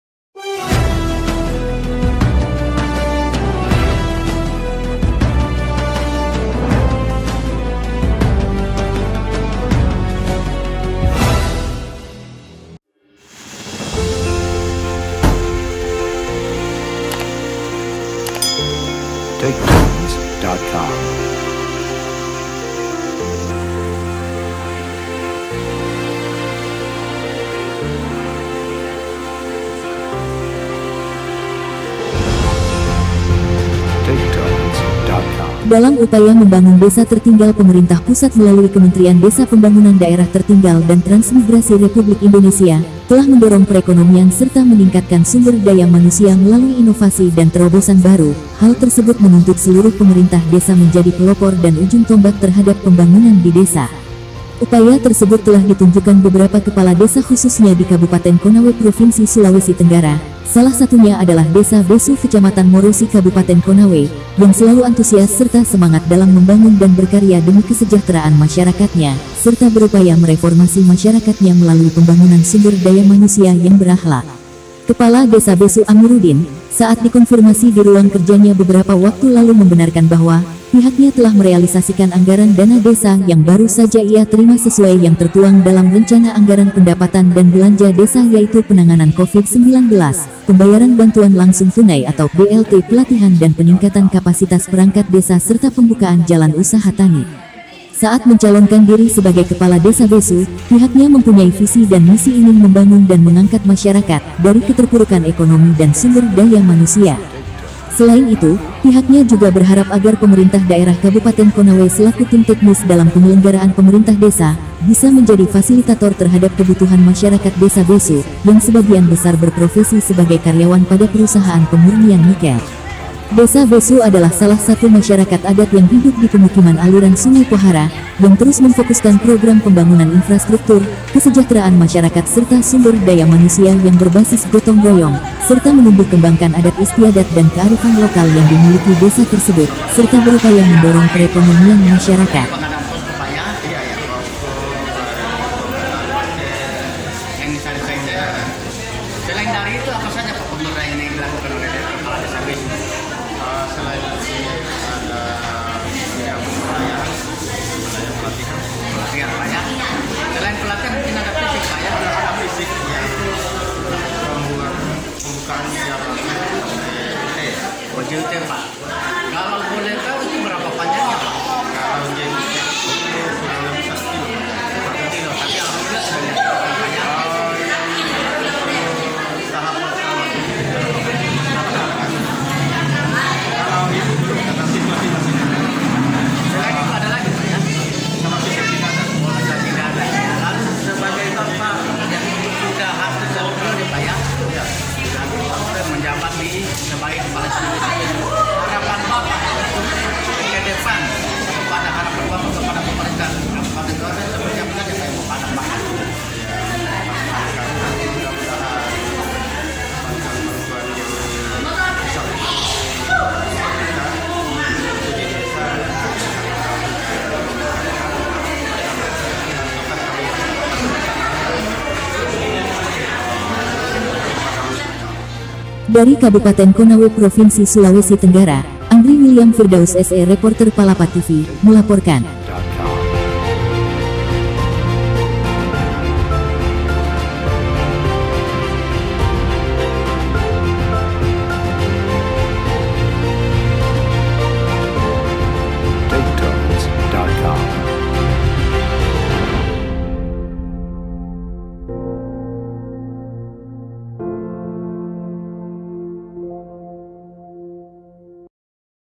PEMDES BESU REALISASIKAN DANA DESA DALAM UPAYA MENDORONG PENANGGULANGAN BENCANA DAN PEMBANGUNAN
Palapa TV,,,,Dalam upaya membangun desa tertinggal pemerintah pusat melalui kementerian desa pembangunan daerah tertinggal dan transmigrasi republik indonesia, telah mendorong perekonomian serta meningkatkan sumber daya manusia melalui inovasi dan terobosan baru, hal tersebut menuntut seluruh pemerintah desa menjadi pelopor dan ujung tombak terhadap pembangunan didesa.
Upaya tersebut telah ditunjukan beberapa kepala desa khususnya di kabupaten konawe provinsi sulawesi tenggara, salah satunya adalah desa Besu kecamatan morosi Kabupaten Konawe, yang selalu antusias serta semangat dalam membangun dan berkarya demi kesejahteraan masyarakatnya ,serta berupaya mereformasi masyarakatnya melalui pembangunan sumber daya manusia yang berakhlak.
Kepala desa Besu (Amiruddin,) saat di konfirmasi diruang kerjanya beberapa waktu lalu membenarkan bahwa, pihaknya telah merealisasikan anggaran dana desa yang baru saja ia terima sesuai yang tertuang dalam rencana anggaran pendapatan dan belanja desa yaitu penanganan covid 19, pembayaran bantuan langsung tunai atau (BLT), pelatihan dan peningkatan kapasitas perangkat desa serta pembukaan jalan usaha tani.